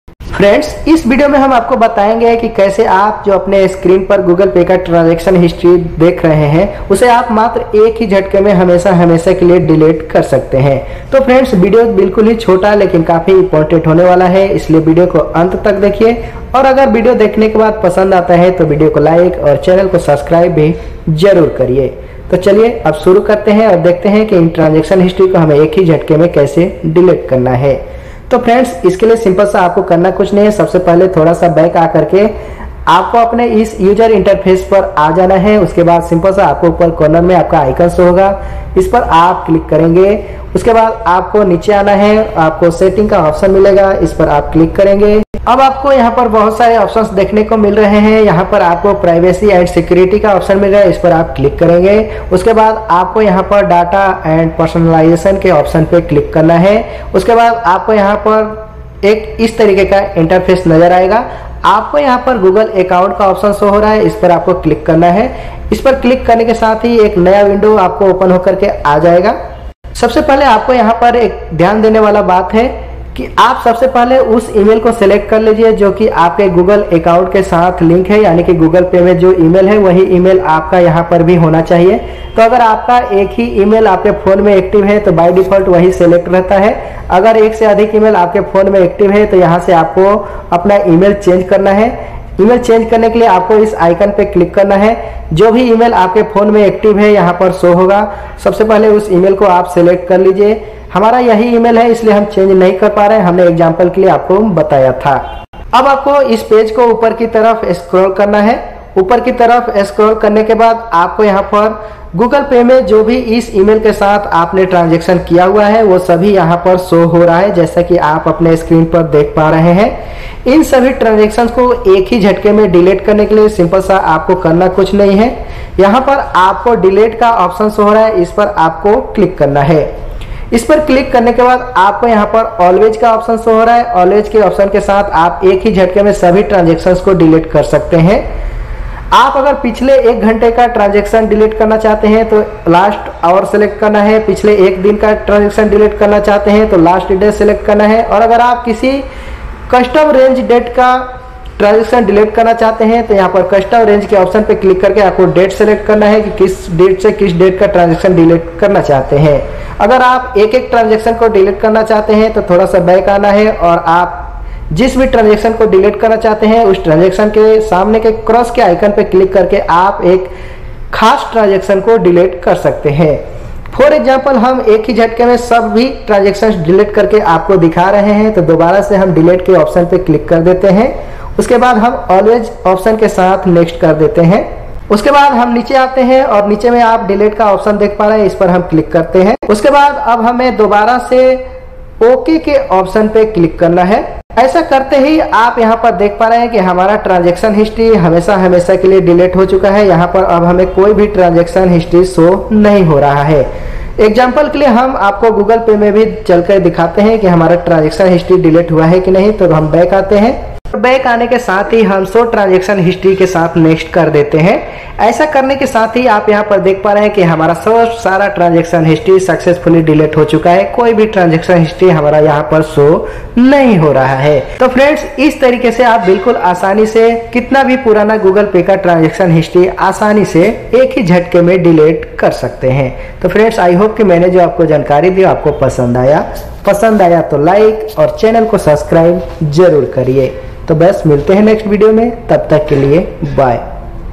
Google pay transaction history delete | Google pay history kaise delete karte hain
Google pay transaction history delete | how to delete google pay transaction history

SUMMARY :

This Video learn Us that How to DELETE Google Pay Transaction history PERMANENTLY.

Please Keep Watching and Give me your Valuable feedback.